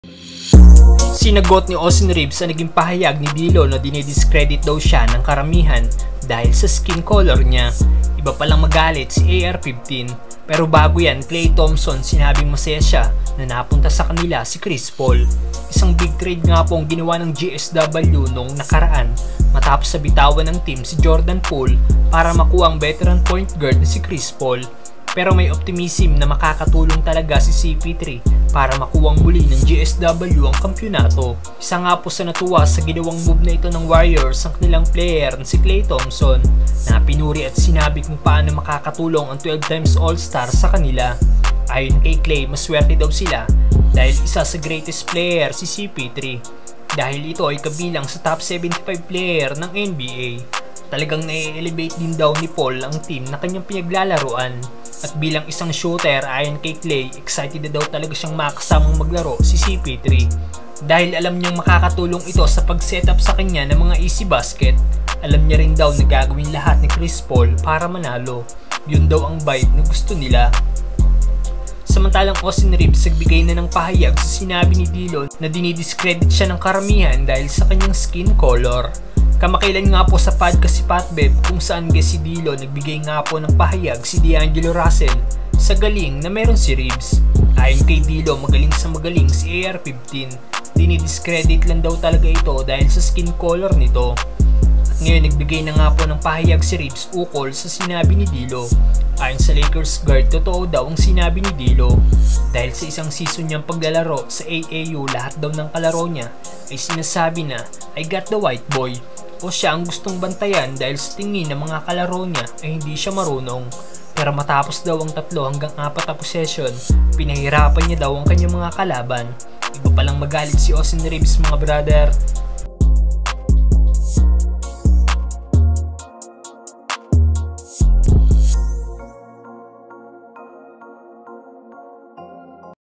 PINAHIRAPAN ni Reaves ang mga nang maliit sa kanya! Klay masaya sa pagkakakuha ni Chris Paul!
LAKERS UPDATE

Related Channel:
Kwentong Atleta
theFastbreak ph
3B Hoops
Hoop News
Basketball update
BASKTBALL TALK
Isportzone